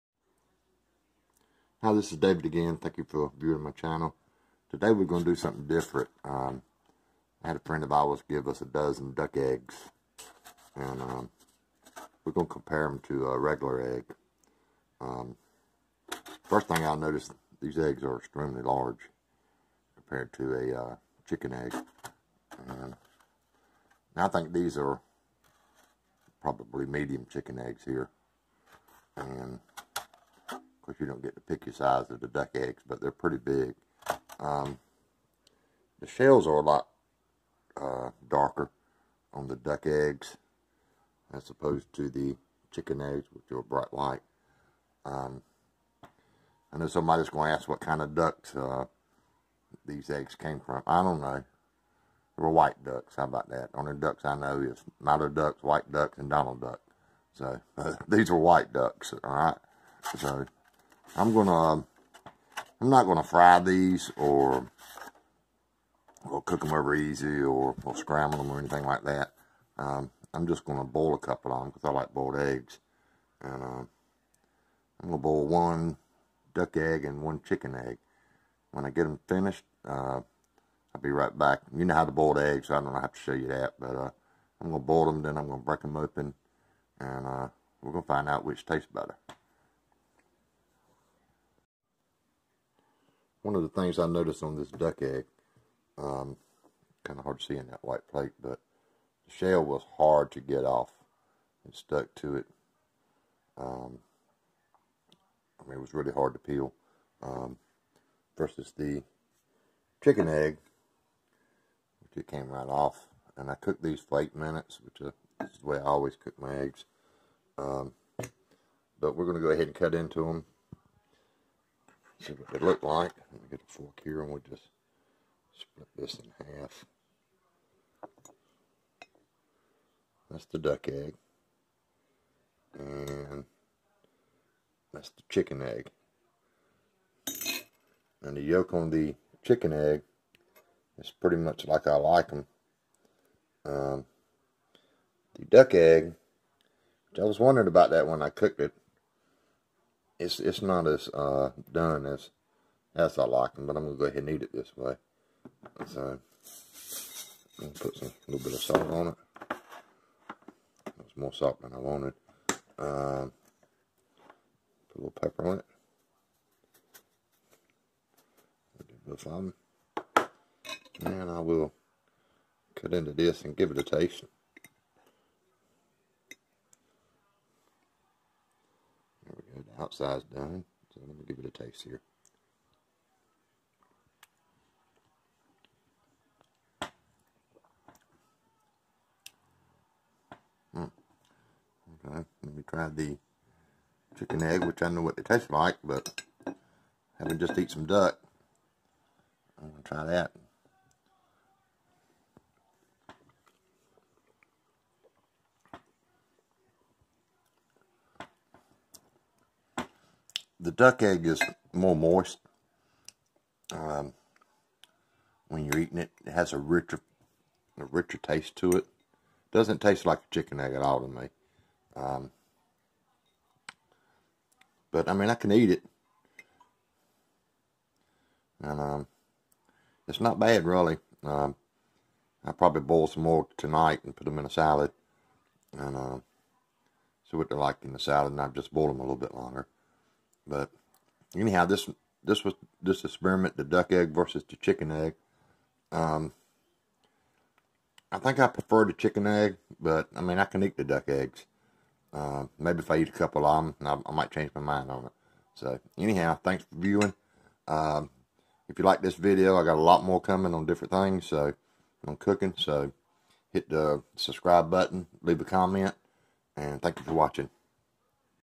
Duck eggs vs chicken eggs which is better.
boiled ducks and chicken eggs